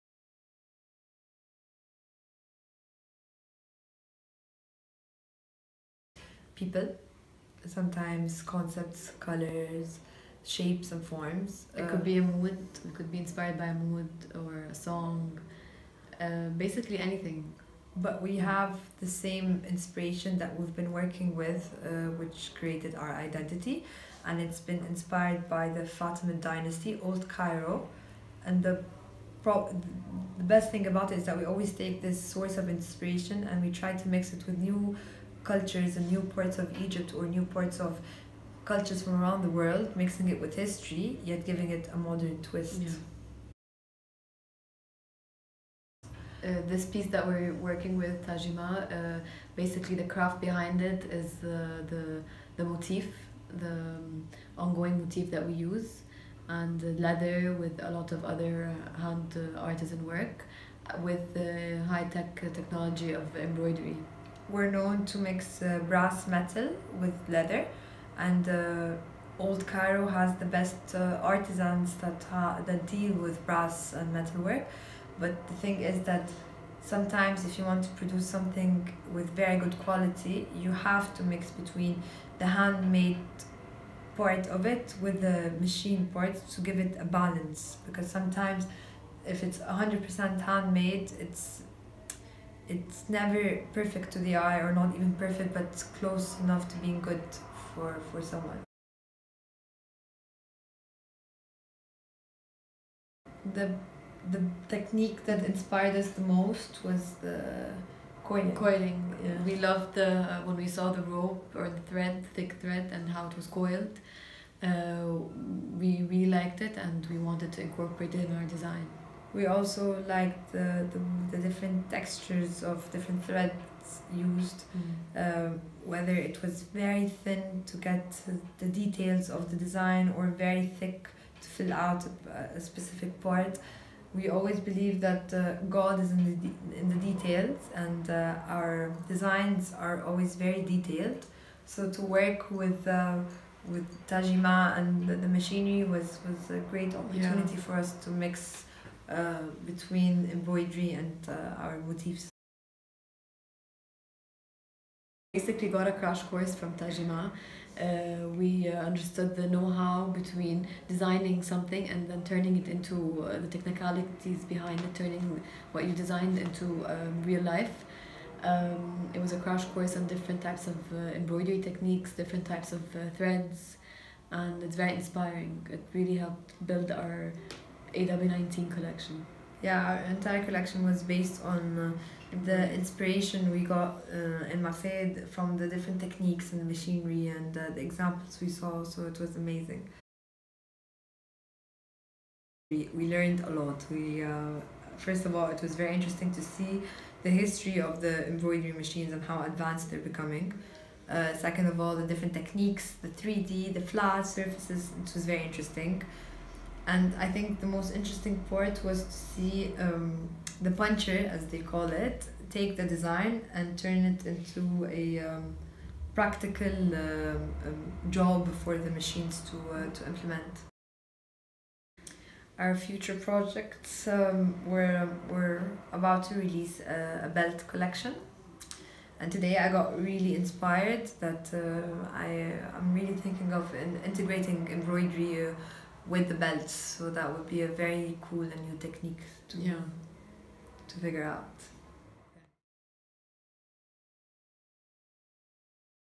[Open My Med Prize] Interview Okhtein - Endowment Tajima
Let's discover Tajima's Endowment with our egyptian OpenMyMed Prize 2017 laureates Aya and Mounaz Abdel Raouf, founders of the label Okhtein.